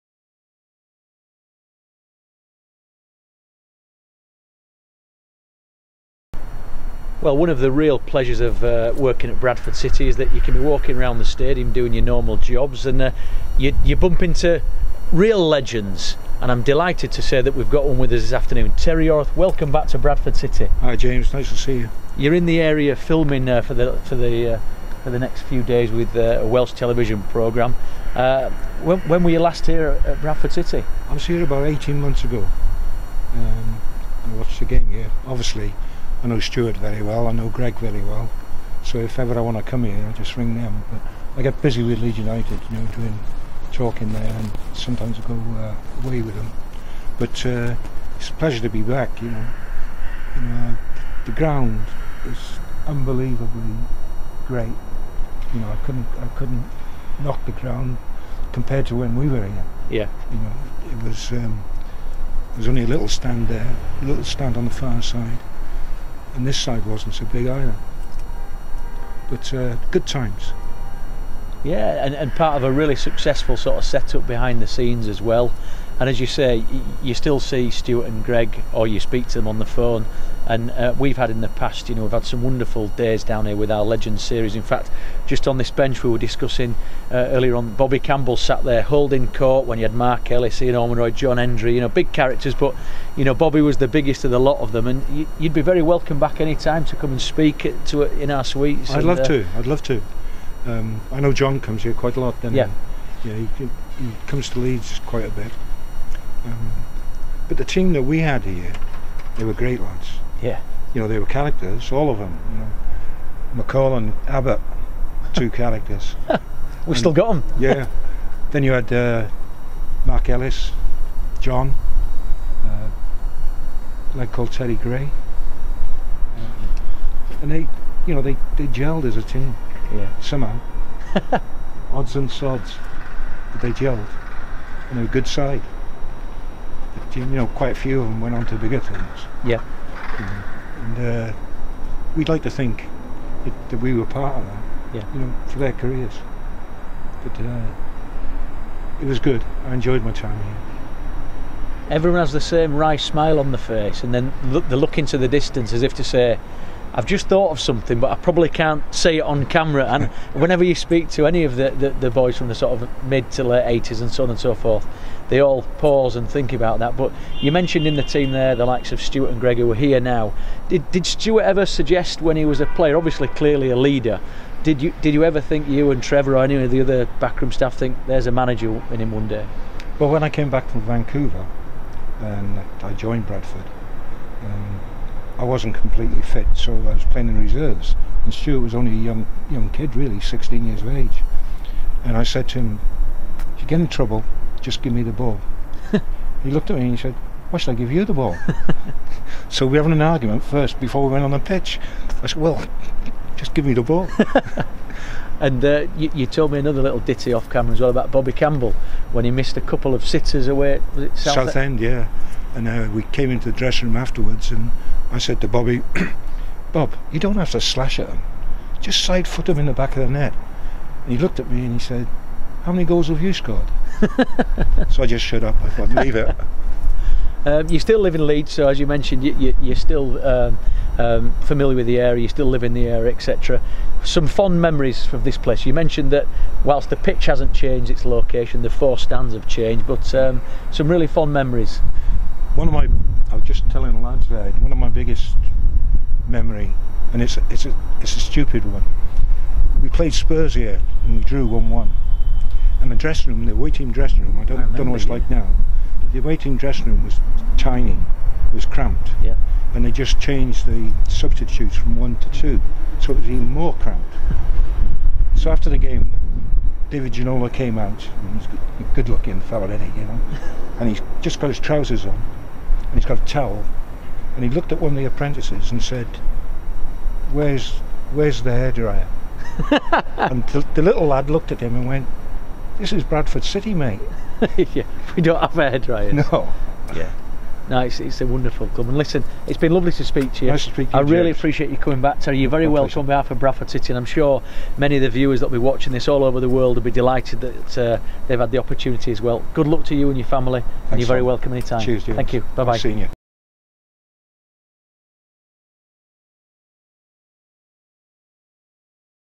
Looking back | A catch-up with Terry Yorath
City's Chief Operating Officer James Mason caught up with Terry Yorath this week after the former Bantams player/manager & coach made a welcome return to the Northern Commercials Stadium